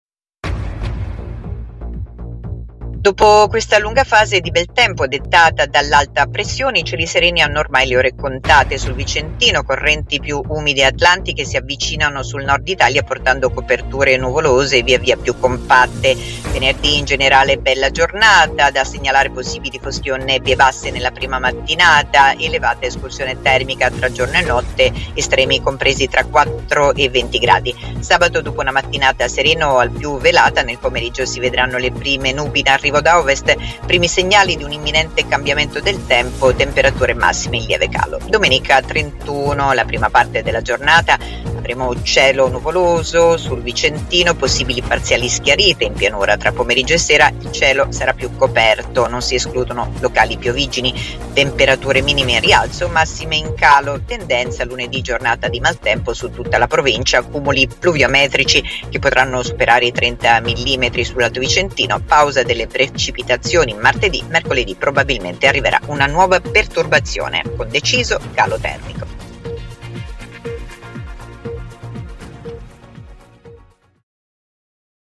Previsioni meteo 29-31 ottobre: ultimi giorni di bel tempo, poi le prime perturbazioni
Dopo questa lunga fase di bel tempo "dettata" dall'alta pressione, i cieli sereni hanno ormai le ore contate sul vicentino. Correnti più umide atlantiche si avvicinano velocemente sul nord Italia, portando coperture nuvolose via via più compatte e le prime precipitazioni.